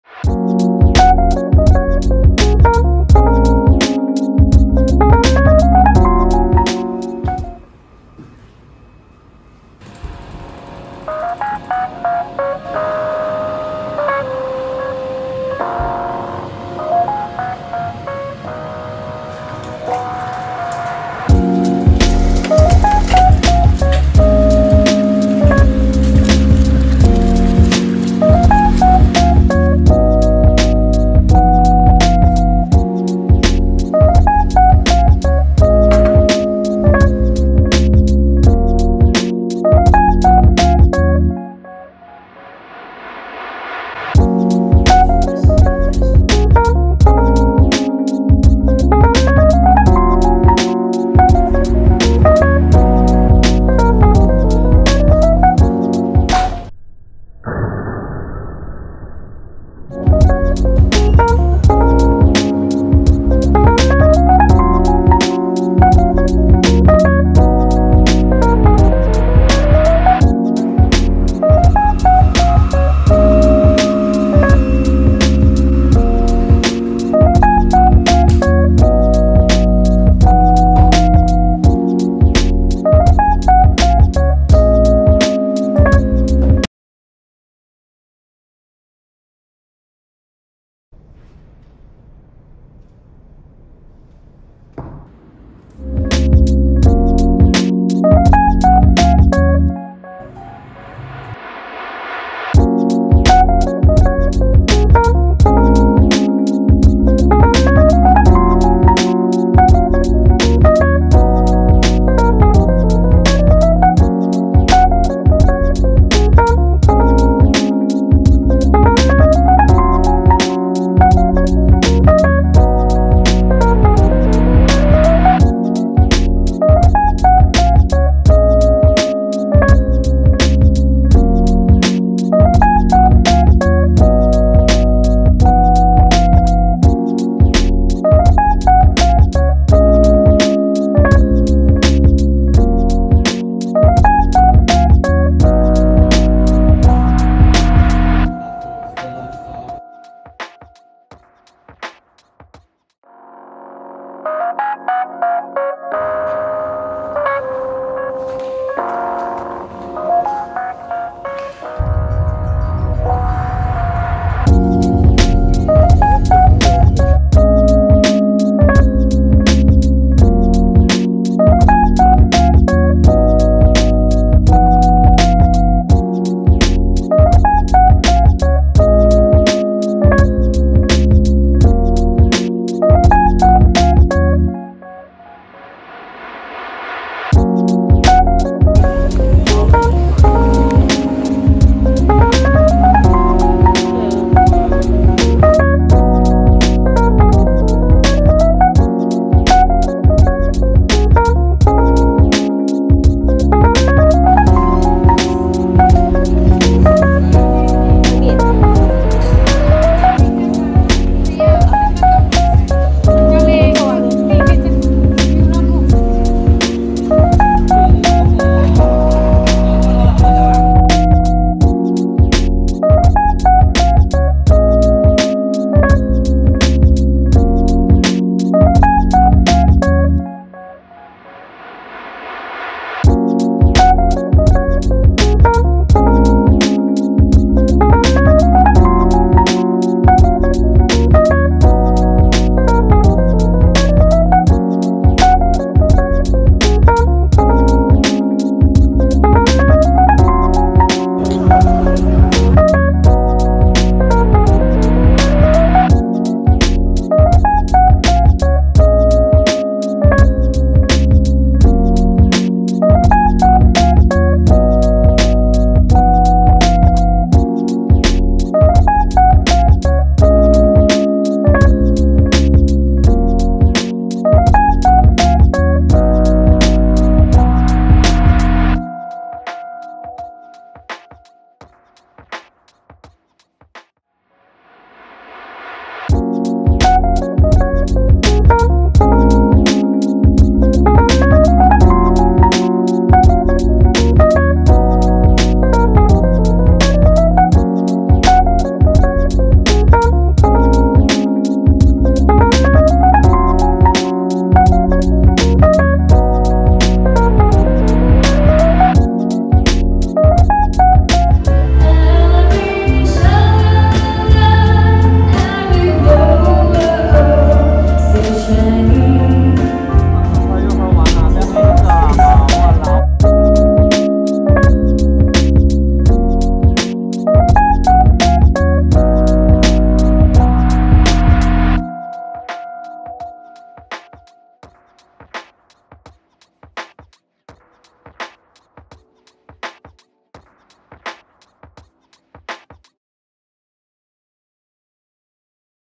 Living Couple's Day Vlog (🇸🇬 +🇲🇾) | House Chores Day, Eating Good Food, Spending Good Times! 🏠🍽️💕
First official YouTube vlog here featuring my boyfriend where I share about our daily life as a couple living together. 🩷